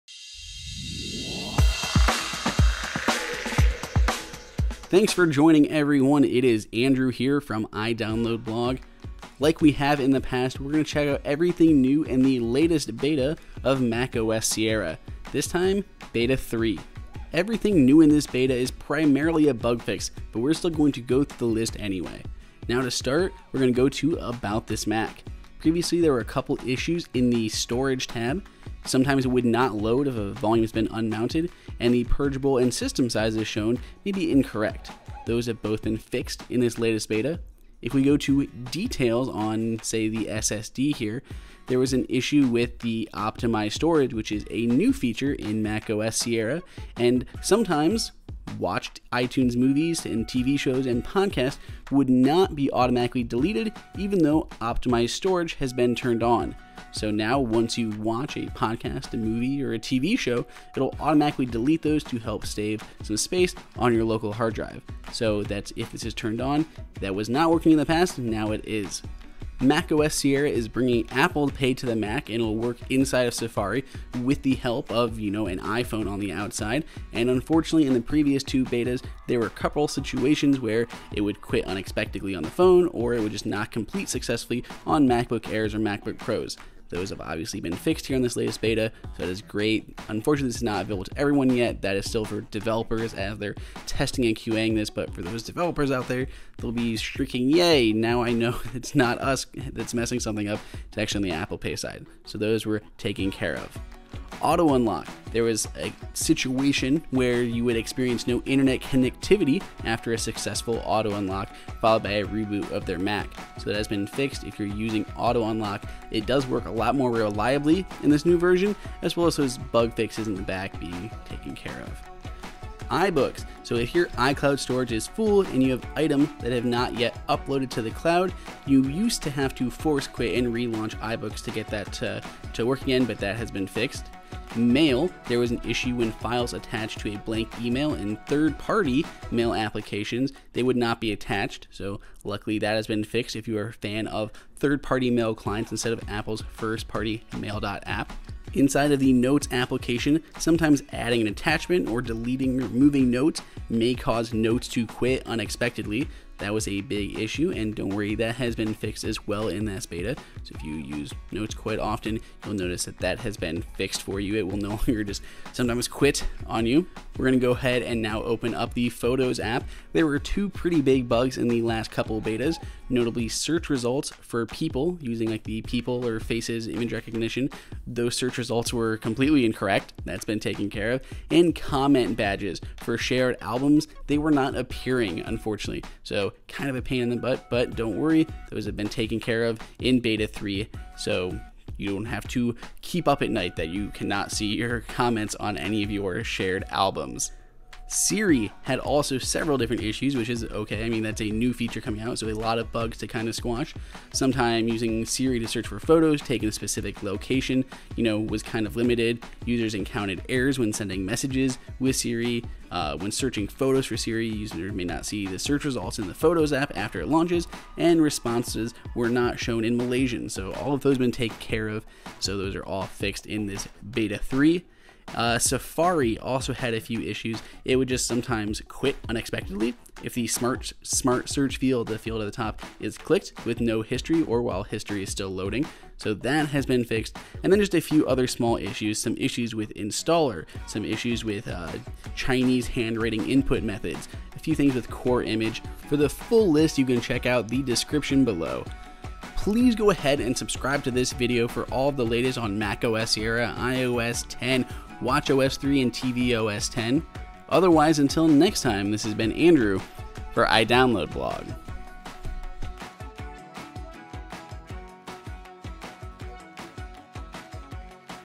Whats New in macOS Sierra Beta 3
Read the article on iDB:
Full Change Log:
About This Mac
The Storage tab in About This Mac may not load if a volume has been unmounted.
The Purgeable and System sizes shown in the Storage tab of About This Mac may be incorrect.
Watched iTunes movies, TV shows, and podcasts will not be automatically deleted even though Optimize Storage has been turned on.
Aperture
On systems with AMD graphics, the system print dialog appears as a black box.
Apple Pay
Creating an Apple Pay for Web PaymentRequest with shipping address + contact will bring up the payment sheet on iPhone but it will quit unexpectedly.
Apple Pay on the web transactions will not complete successfully on MacBook Air and MacBook Pro.
Auto Unlock
Users may experience no internet connectivity after a successful Auto Unlock, followed by a reboot of their Mac.
Back to my Mac
Back to my Mac cannot be started for some users.
CoreImage
The input and output data for the CIImageProcessor API is now ordered so the the upper-left-most pixel is the first byte in the buffer.
iBooks
If your iCloud storage is full, and you have items that have not yet uploaded to the cloud, you need to force quit and relaunch iBooks in order to proceed with a storage upgrade.
Input Methods
Using the Trackpad Handwriting feature for Chinese may not work reliably when using trackpads that do not support Force Touch.
When using the Pinyin input method to type Chinese, uppercase typing incorrectly produces full-width Latin letters. Additionally, some of the keys may not produce the expected punctuation symbols.
Installer
Recovery install fails when Safari is used in Recovery environment.
Systems with FileVault enabled may hang at the login screen after updating.
Mail
Files attached to a blank email from 3rd-party applications may not be attached.
Notes
Adding an attachment, or deleting or moving notes may cause Notes to quit unexpectedly.
Photos
Search results for people are incorrect.
Comment badges may not appear in Shared Albums.
Safari
Safari may quit unexpectedly if the Smart Search field is clicked with no History or while History is still loading.
Siri
Users may encounter errors while sending Messages with Siri using relationships or during contact disambiguation.
Using Siri to search for Photos taken in a specific location has limited support.
When searching for photos with Siri, users may not see search results in the Photos app after it launches. Responses are not supported in Malaysian.

About iDB